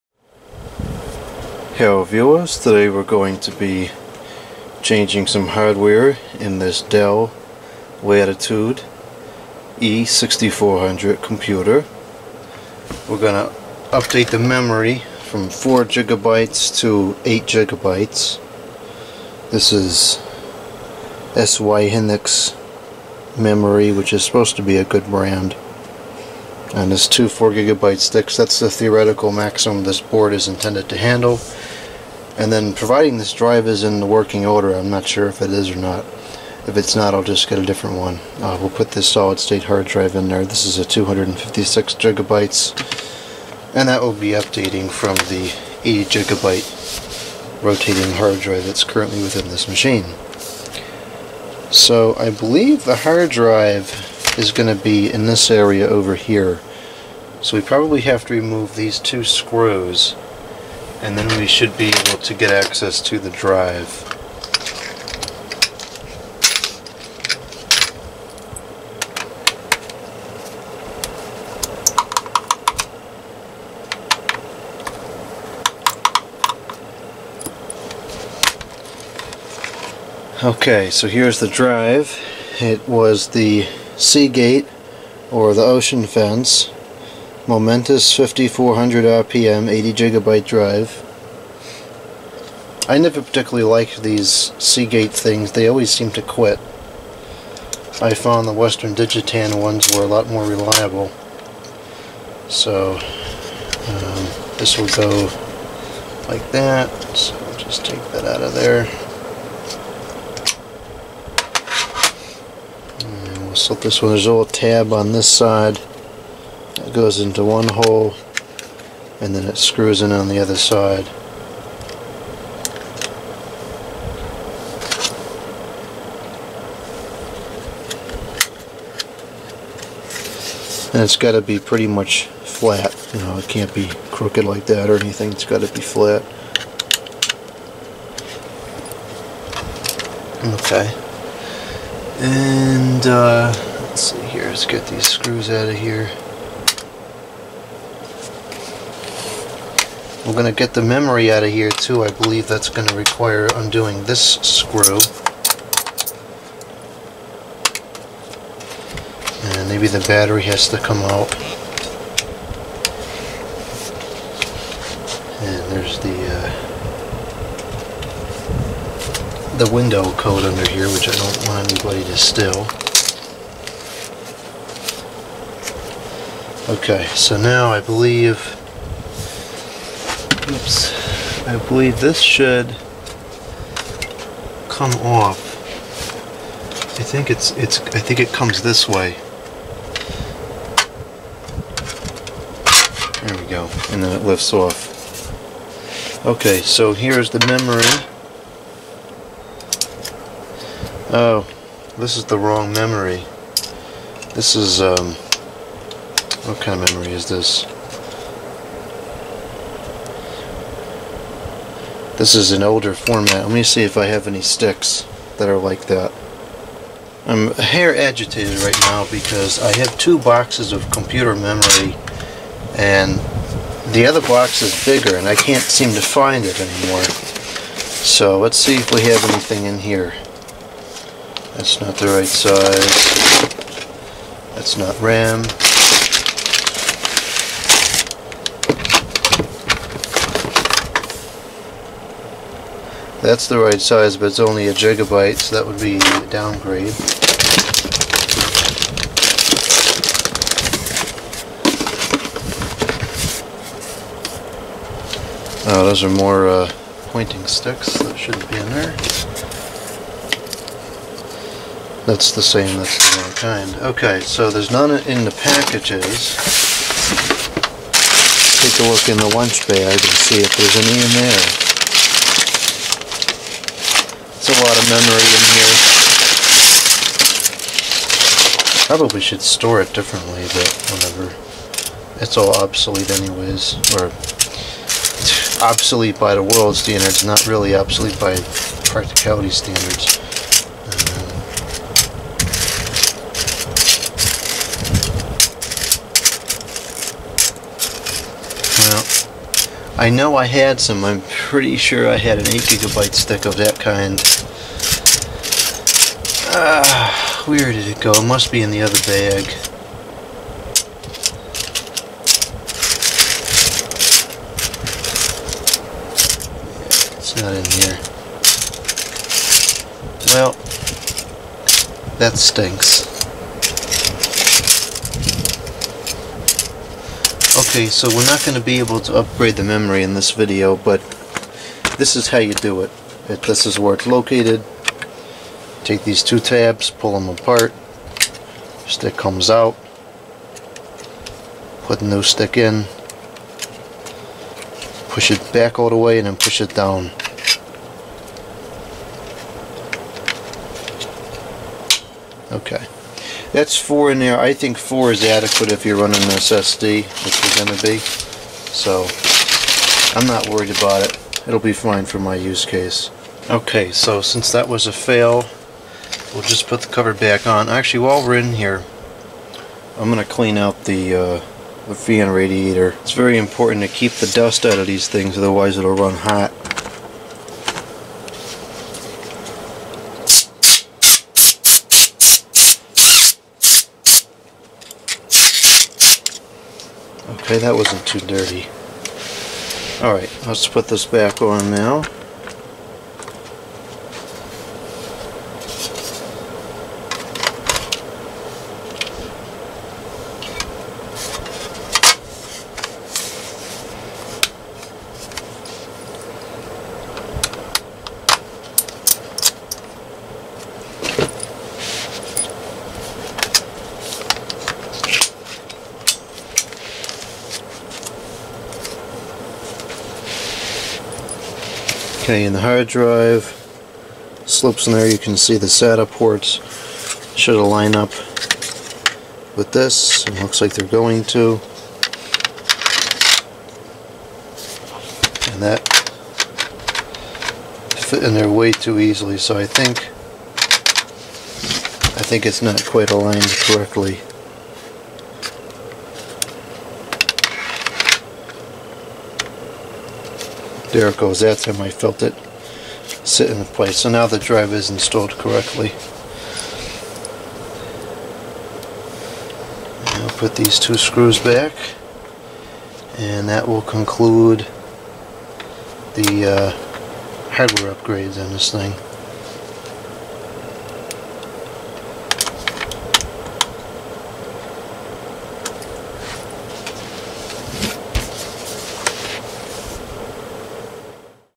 Dell Latitude E6400 Memory and Hard Drive Upgrades
How to replace the memory and the hard drive in a Dell Latitude E6400 laptop computer.